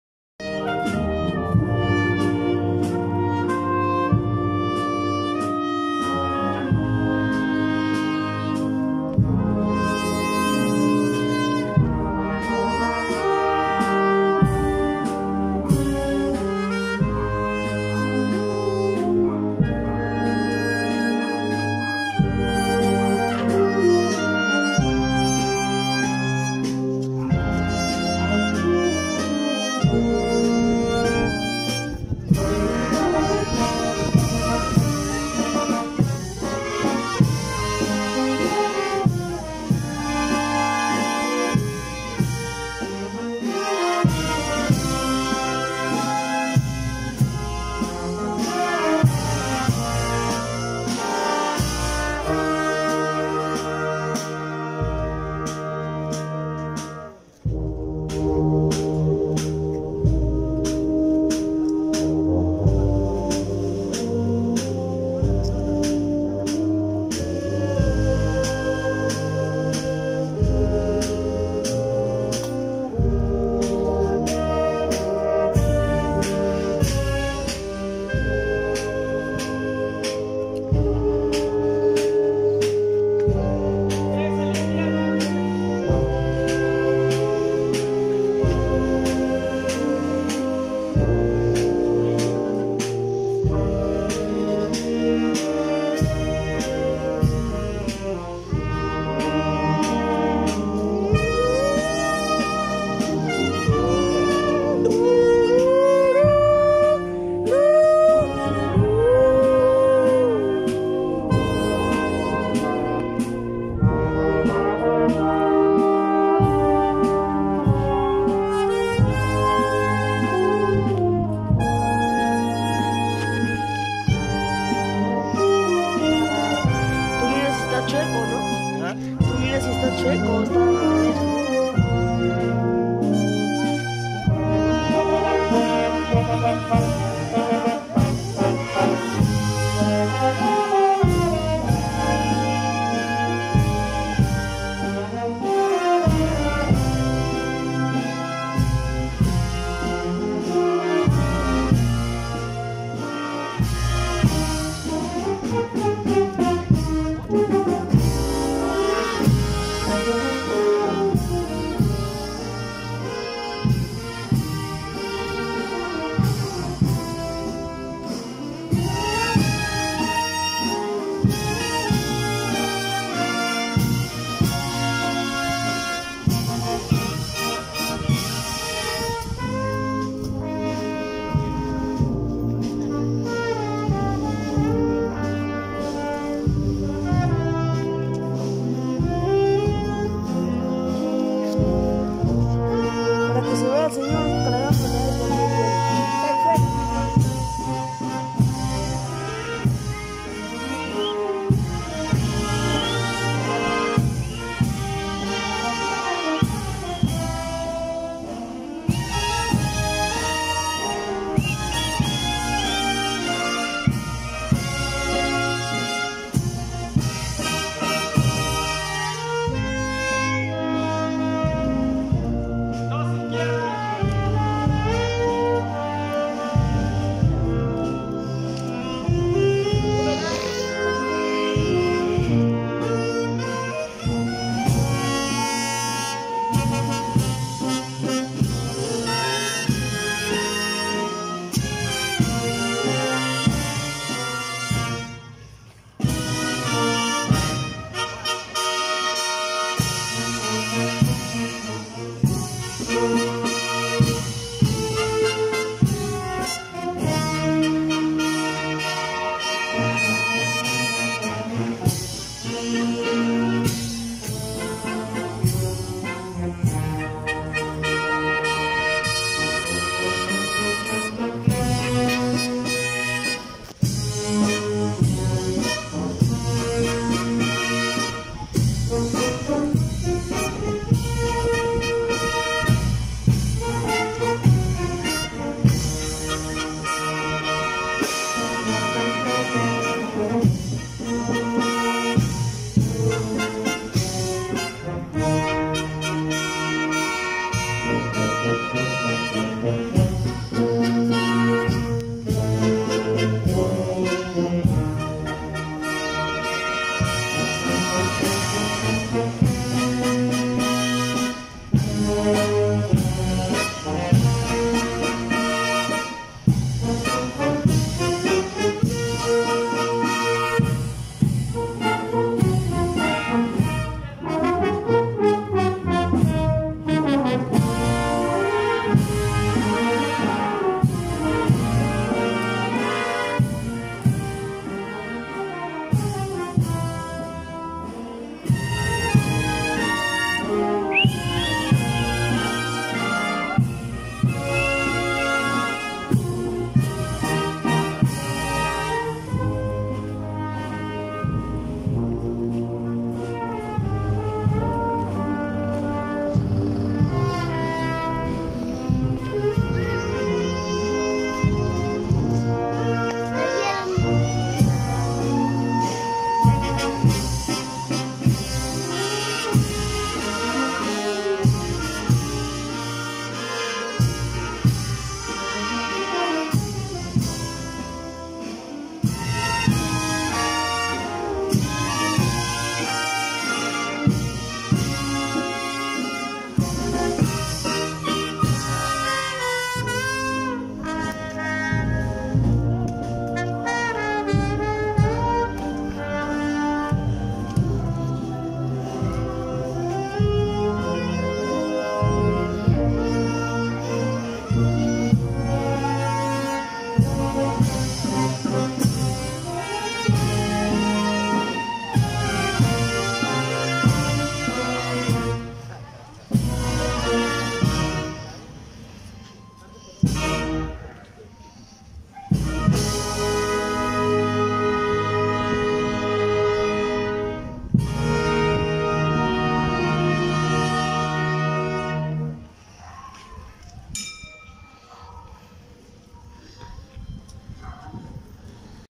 Recorrido Procesional - Festividad de Octava de Huaura 2022
"Señor de  de Huaura" - "OCTAVA 2022"
Acompañamiento Musical: Banda  "Son en Clase"